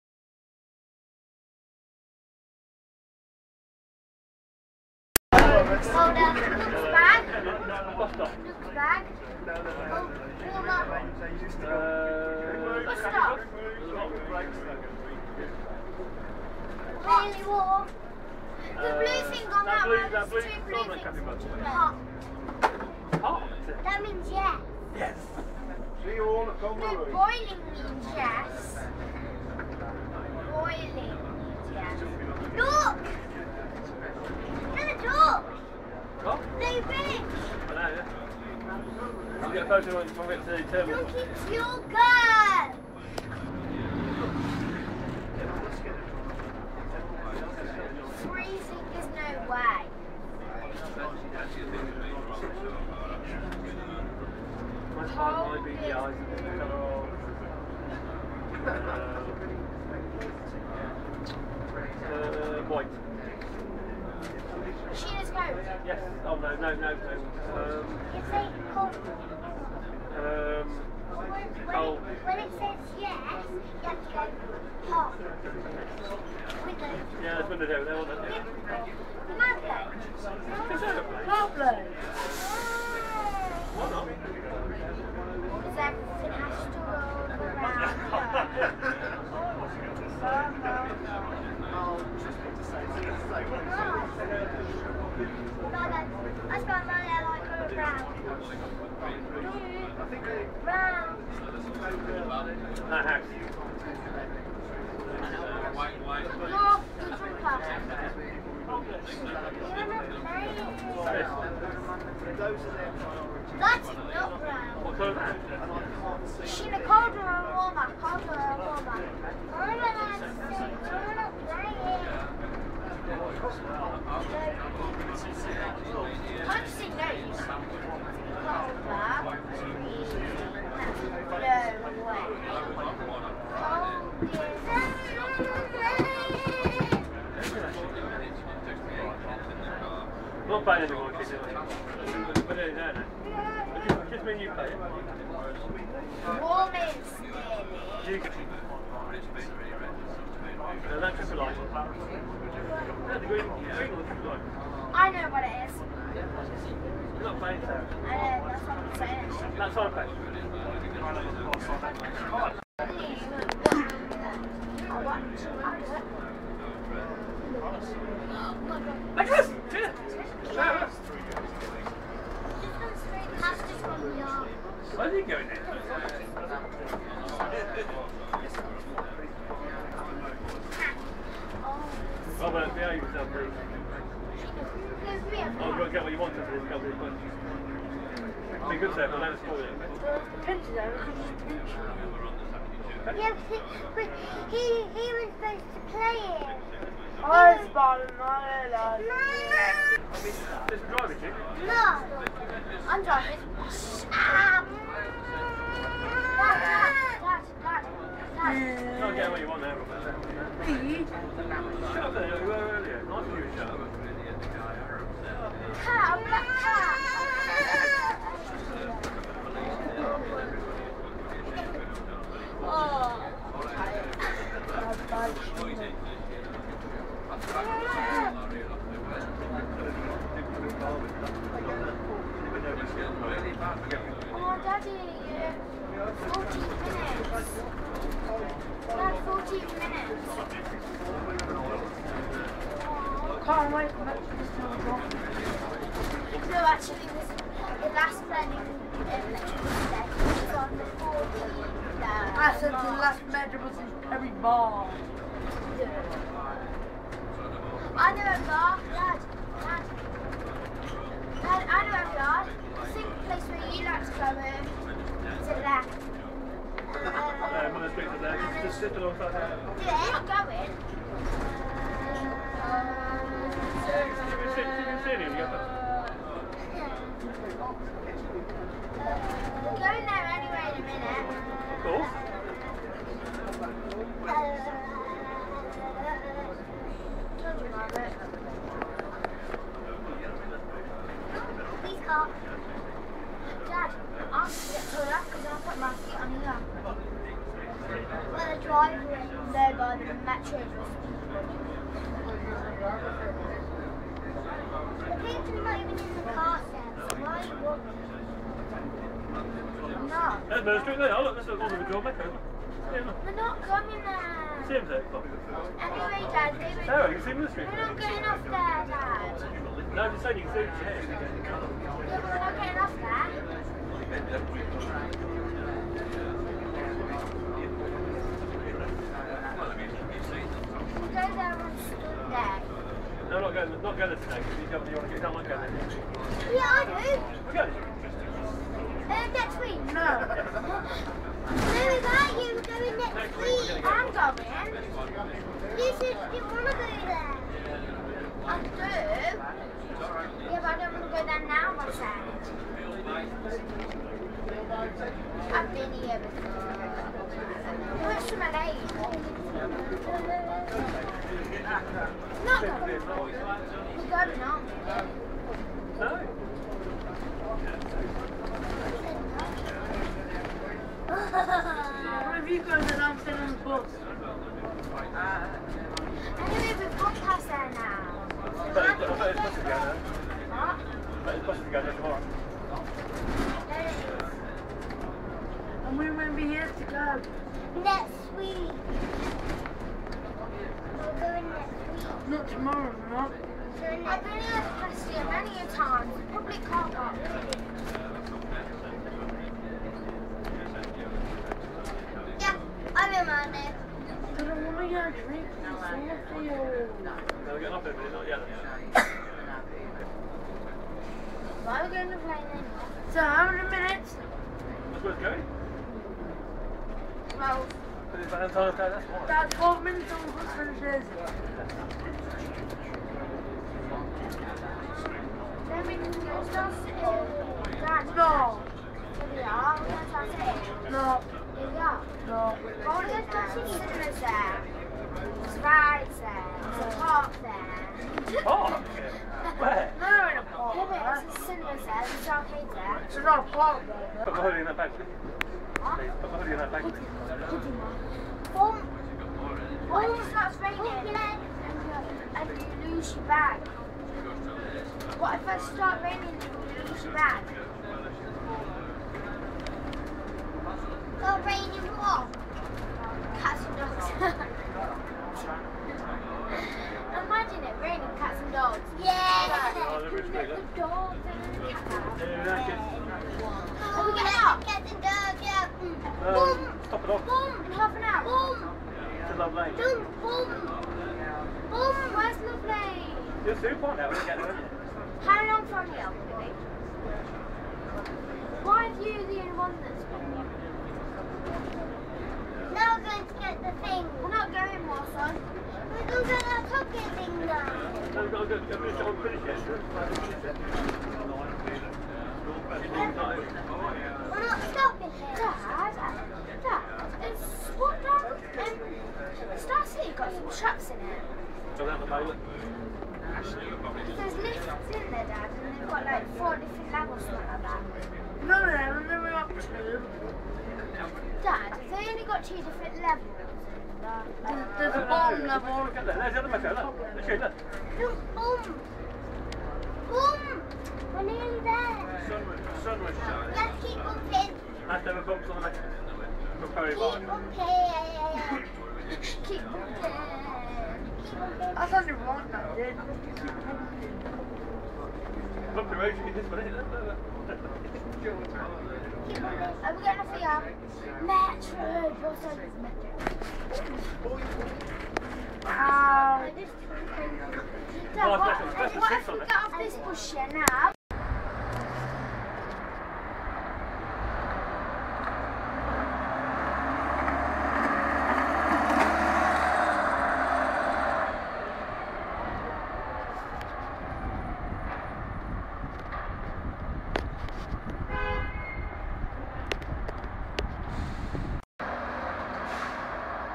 Roxys Final Fling
On Valentines Day, 14th February 2010, National Express West Midlands operated the last MCW Metrobus in Schedueled service from Perry Barr Depot, 2654 ROX654Y (otherwise known as Roxy) was held at the depot so as to be able to perform three round trips onthe No 7 route from Perry Commom to Birmingham City Centre, this short cobbled together video was filmed on the final run to Colmore row and ends with 2654 departing for a farewell photoshoot at Love Lane.